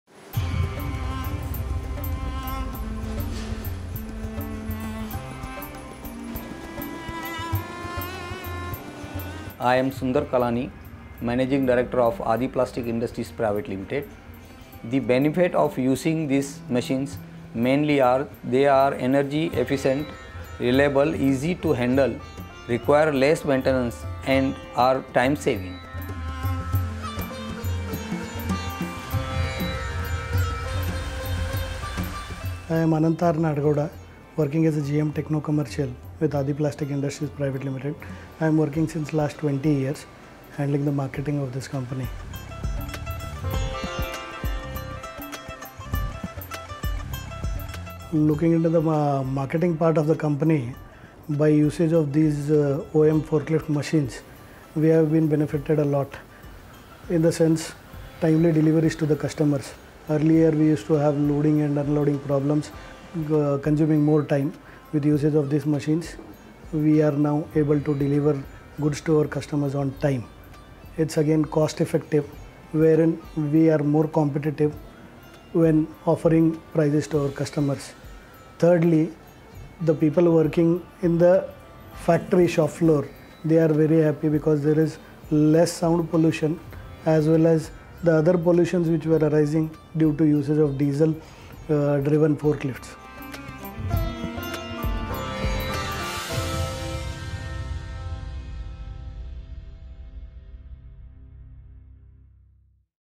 OM XE 20- 2Ton Electric Forklift | Customer - Aadi Plastics Industries Private Limited
KION India with its OM brand has launched it's Electric 2Ton Forklift in India. We show you how our esteemed customer have used the forklift and have found value in everyday use.